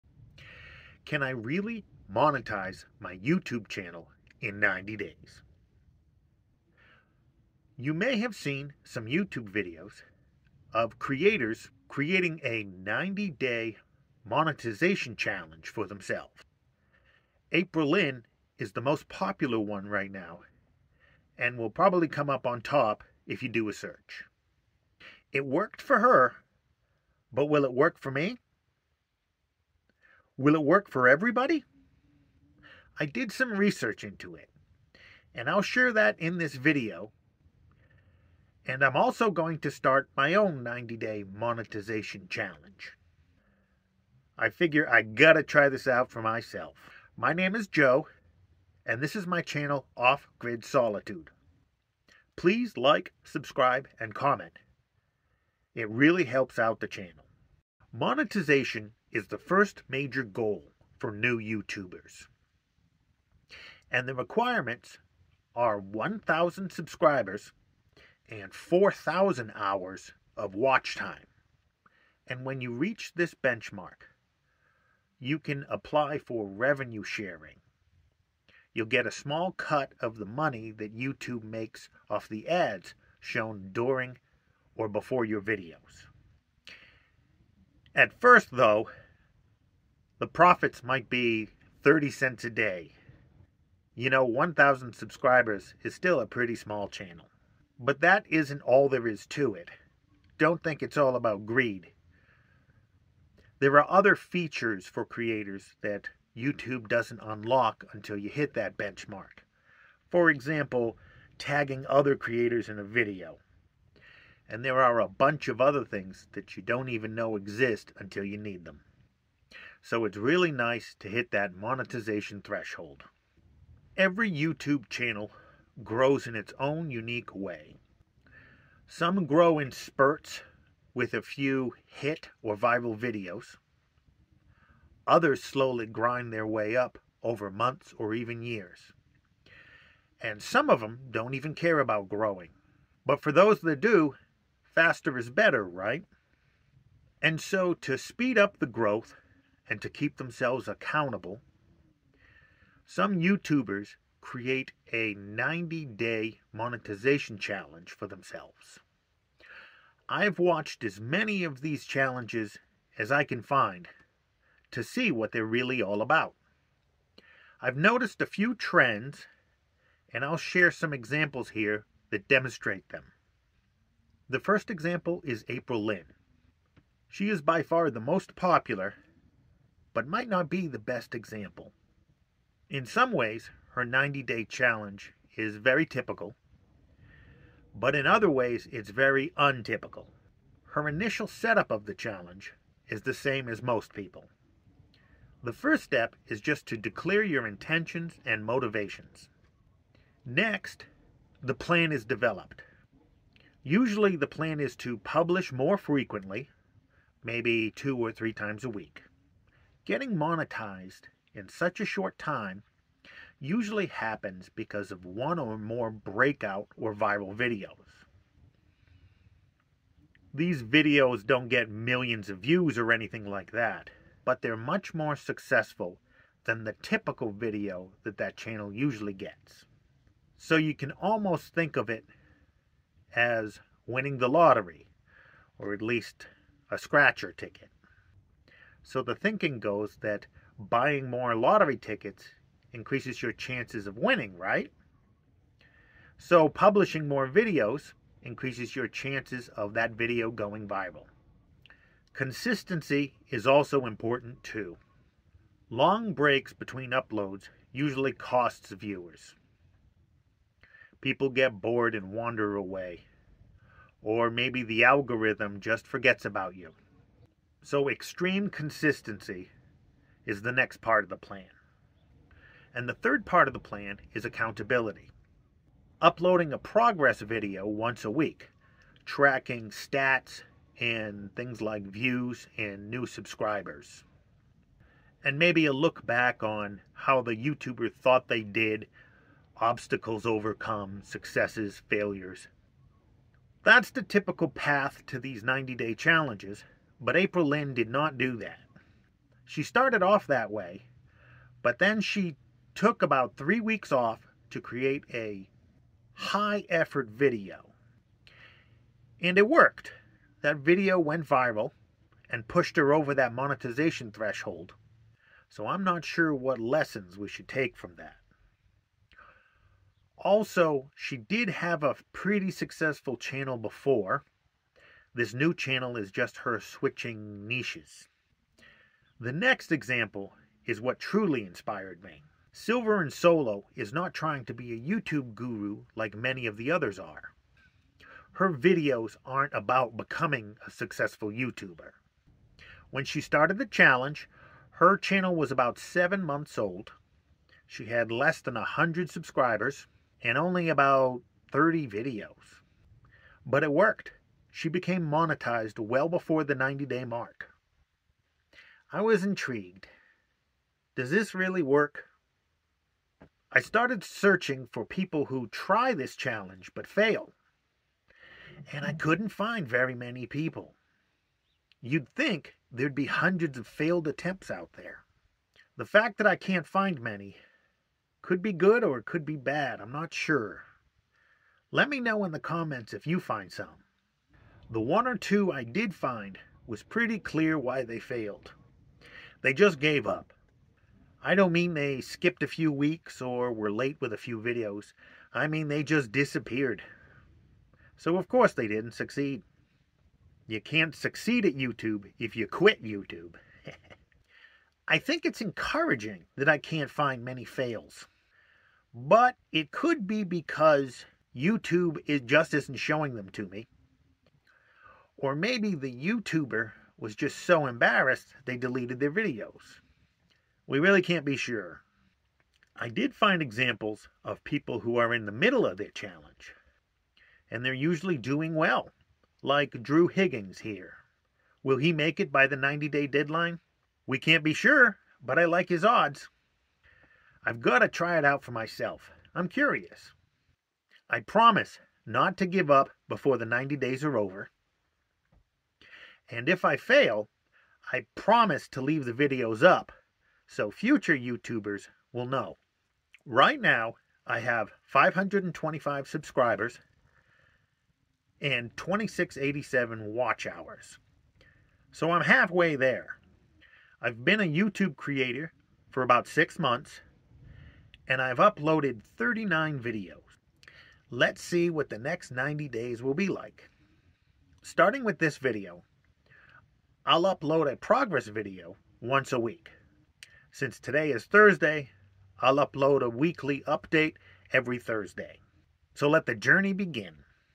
Can I REALLY Get Monetized in 90 Days?
I am starting a 90 day Youtube Monetization Challenge.